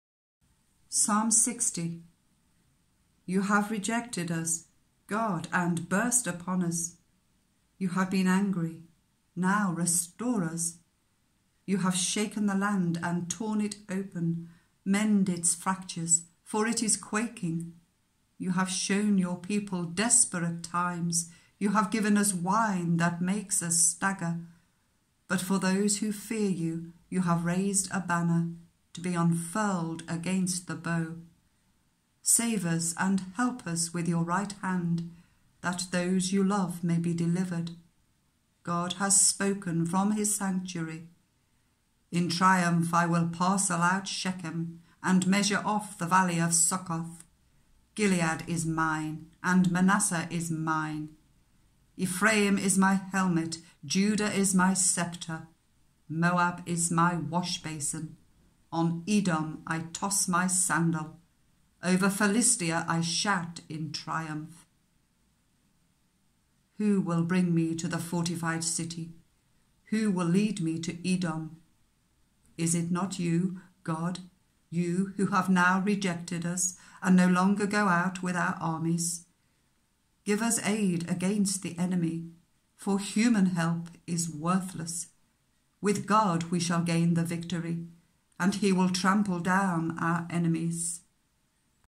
Psalm 60
Audio Bible: TNIV
The 66 Books of the Holy Bible, narrated by Gayle Grieves. Individually recorded chapters for listening and downloading